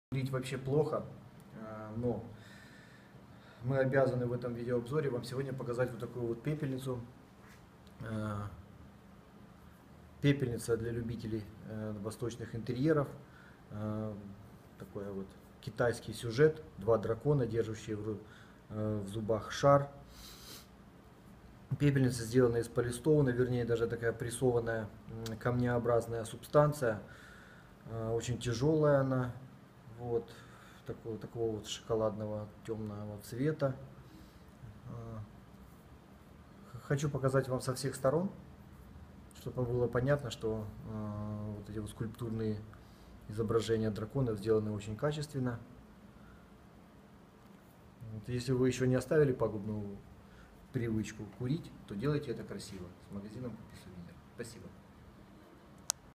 Пепельница «Два дракона»
Сразу два в одном — и пепельница, кстати, очень удобная и практичная, и в то же время — целый кладезь благоприятнейшей символики. Два дракона, непрестанно дерущихся на крыше мира за право обладания волшебным артефактом. Этот сюжет встречается довольно часто во всех восточных культурах, и означает он обычно что-то позитивное и благоприятное.